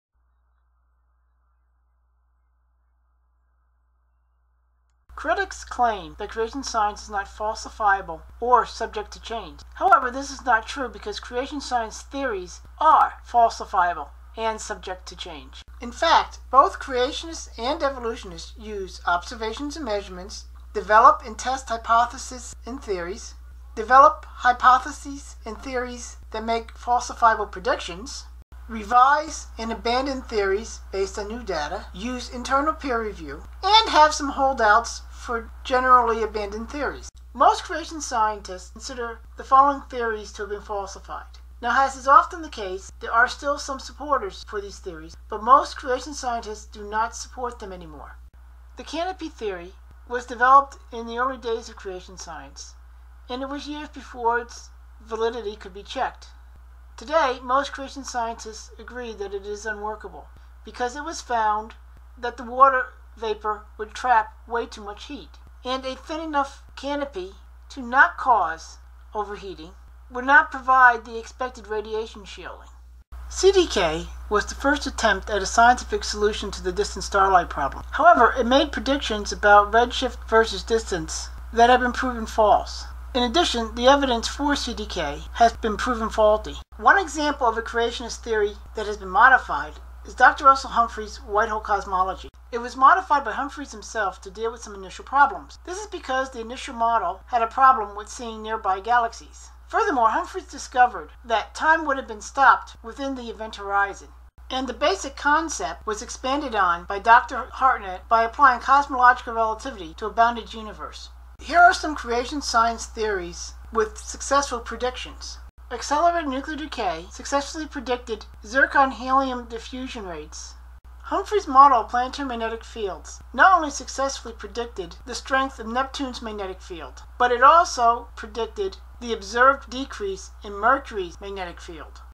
Is Creation Science, Science? I
This video is a discussion of the scientific nature of Creation Science. It examines why creation science qualifies for science. Furthermore, the claims that it is not are based on a misrepresentation about science in creation science.

The Answers Book

The New Answers Book 1

The New Answers Book 2

The New Answers Book 3